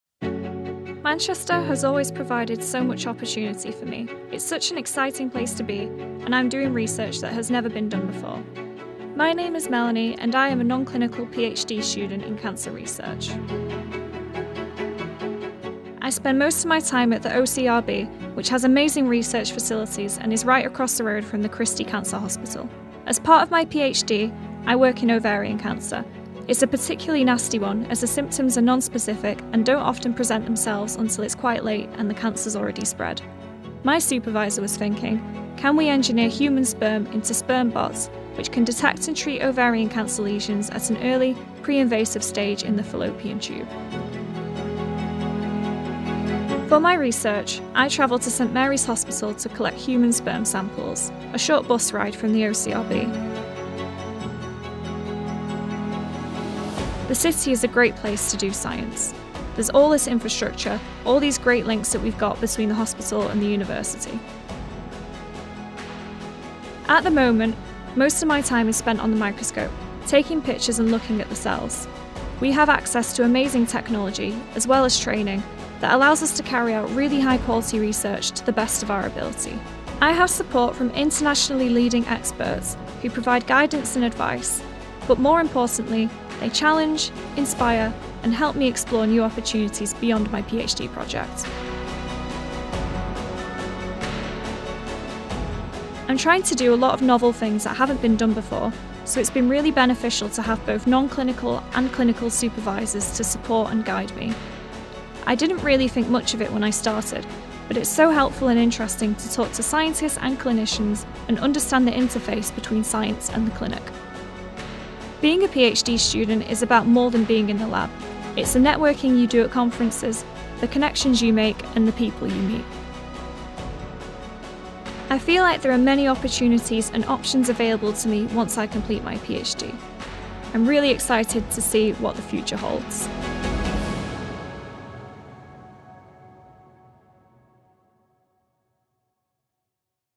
A Day in the Life of an MCRC Non-Clinical PhD Student | Melanie Seaton
Melanie is a non-clinical PhD student in cancer research. This 'Day in the Life' gives us a glimpse into what it is like to be a PhD student in Manchester.

Read more about PhD training and opportunities on the CRUK Manchester Centre